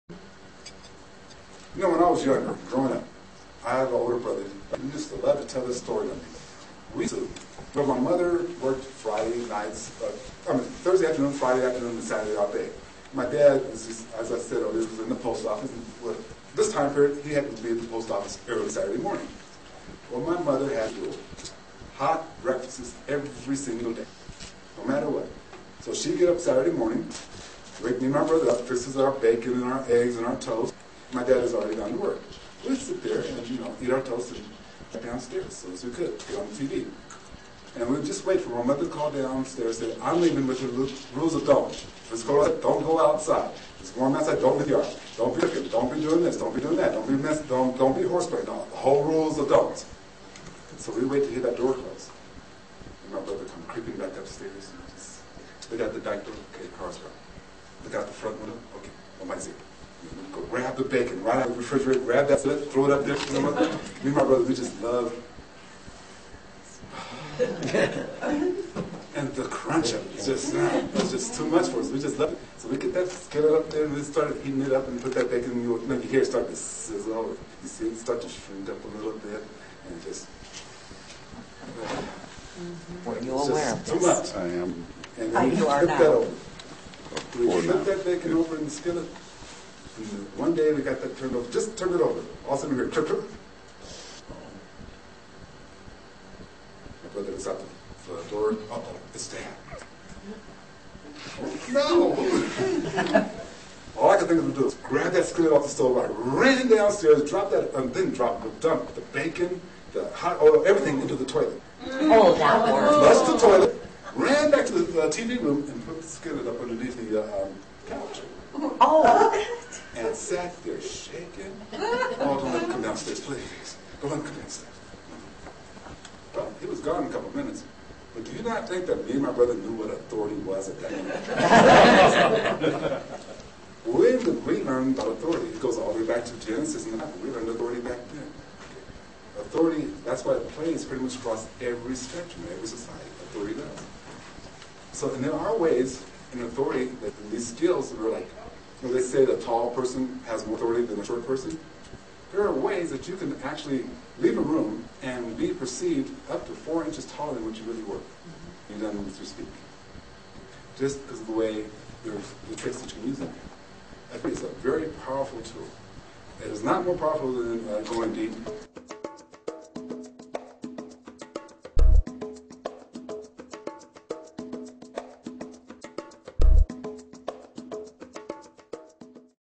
Authority and the Power of Bacon
The Science of Analytical Human Behavior
The study of how we make our buying decision and how we can control that process.